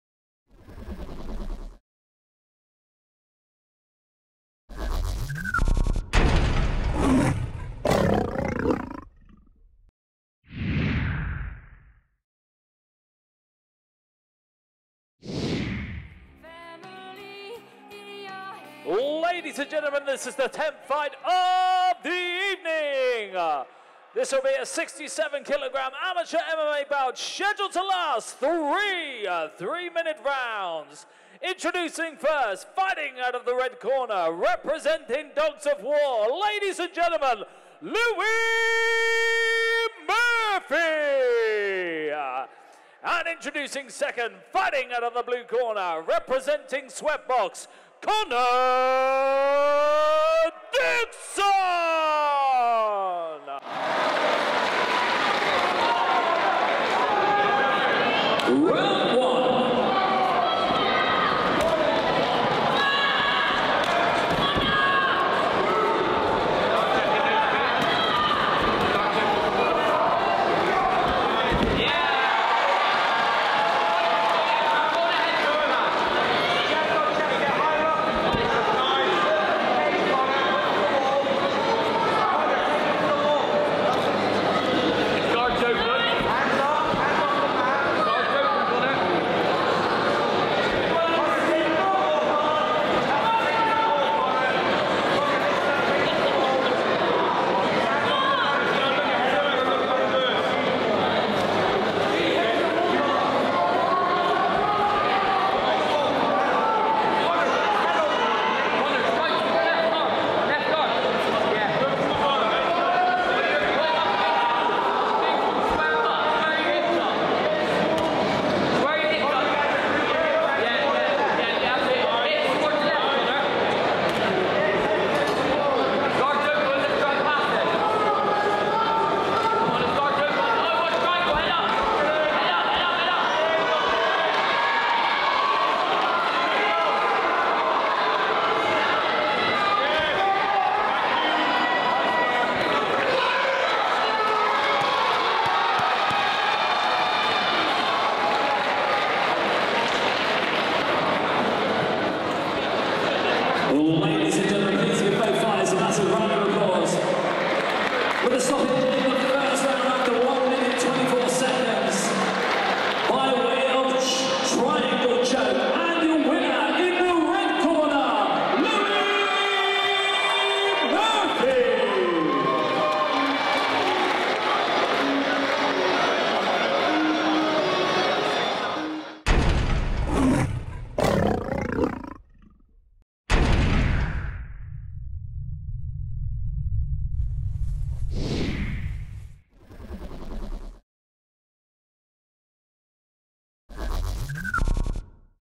Caged SAS 10 | Fight 10 | LOUIE MURPHY v CONNOR DIXON
Caged Stand and Strike 10
27th January 2024
Hutton Moor
Weston-Super-Mare